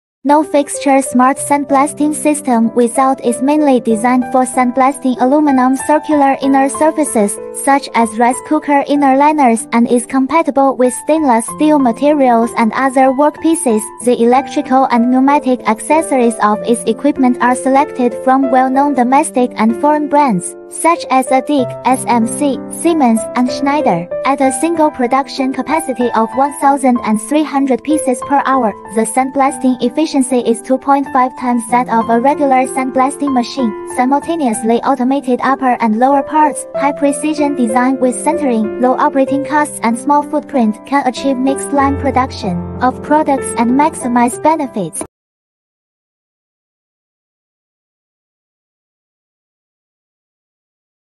Fixture-Less Intelligent Sandblasting Equipment
The production capacity of a single equipment is up to 1,000 pieces per hour, it occupies a small area, has a high degree of automation, and can realize mixed line production.

Add: No.2, Yingbin Avenue, High-tech Zone, Zhaoqing City, Guangdong Province, China